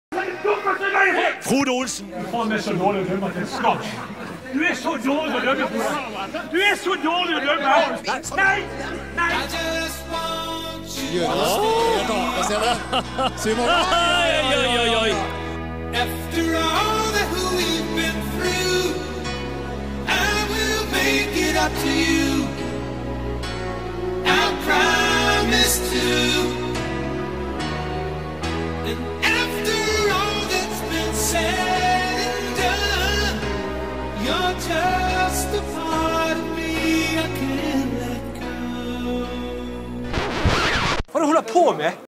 Davy Wathne grisetakler Frode Olsen
Davy Wathne grisetakler Frode Olsen på TV 2 Sportens internkonkurranse før sesongstart 2009